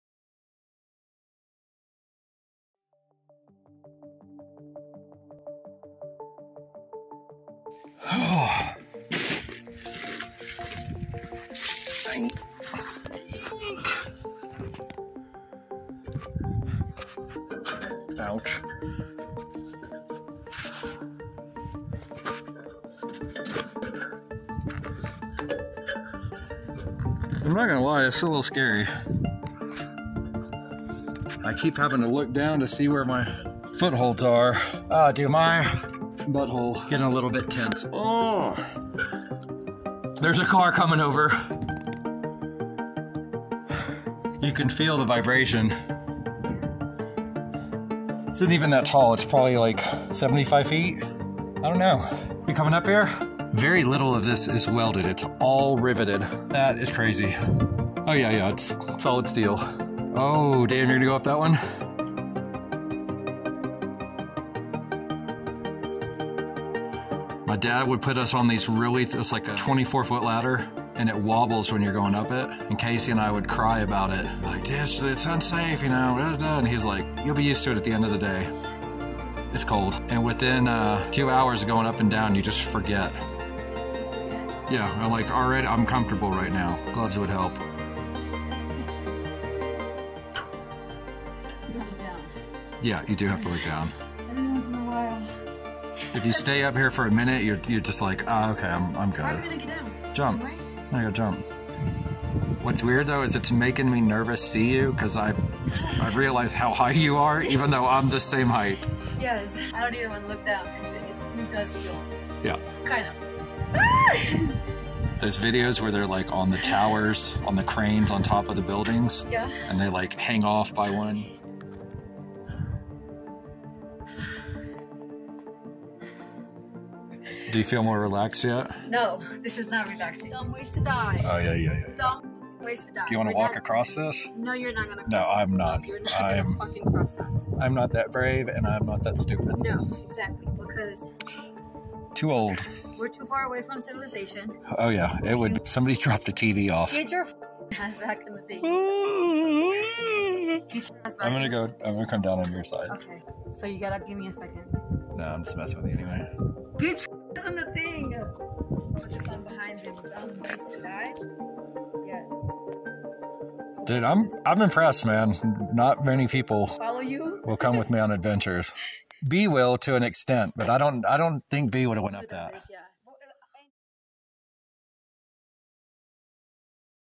Climbing an old bridge?!?!
Today, Karina and I climb an old bridge in the middle of nowhere.  Every day is an adventure.  It's really not.  Most days are boring, so I live for these little adventures.